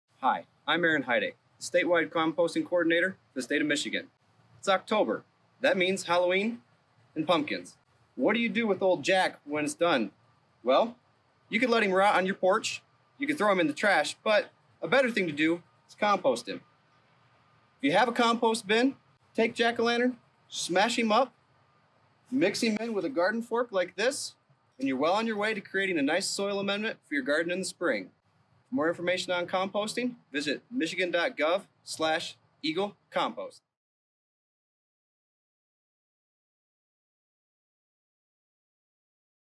Compost Your Jack-O'-Lantern!
Every year millions of pounds of pumpkins end up in landfills after Halloween, decomposing without oxygen and creating methane gas--which not only stinks, but is also contributes to global warming. Luckily, Aaron Hiday has the perfect solution for your ghostly gourd!